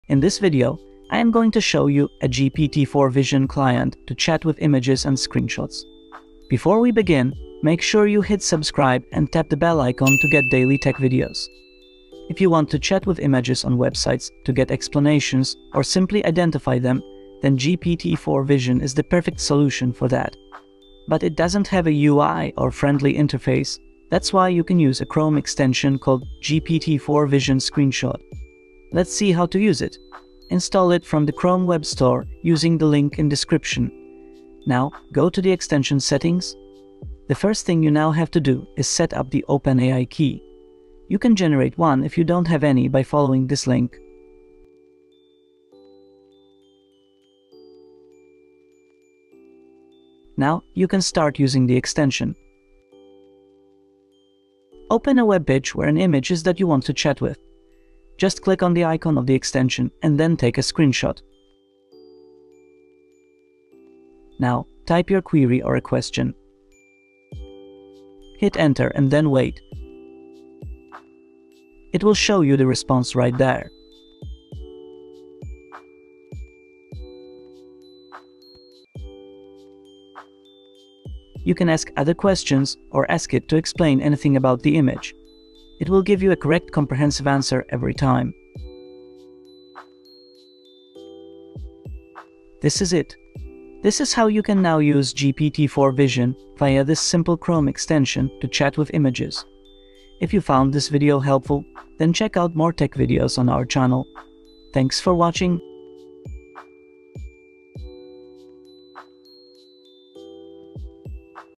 GPT-4 Vision Client to Chat with Images and Screenshots
Unlock image insights with GPT-4 Vision! 🔍 In this video, I'll show you how to chat with images and screenshots using the GPT-4 Vision Screenshot Chrome extension. 🔑 Then, take a screenshot of an image on a webpage and ask your question. 💬 GPT-4 Vision will provide comprehensive answers. 🤔 Ask follow-up questions or request image explanations. 📝